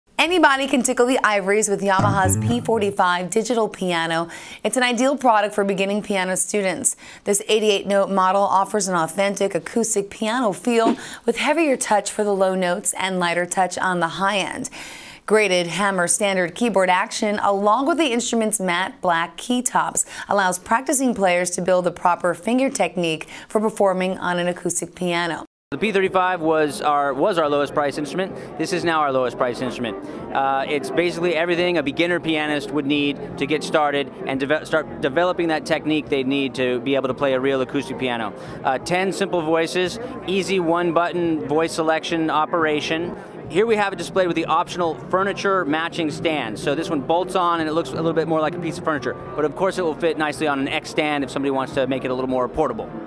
Yamaha P45 Digital Piano
The Yamaha P45 Digital Piano is the newest member of the Yamaha P Series pianos. It has a contemporary design with a small footprint that allows for easy portability and storage.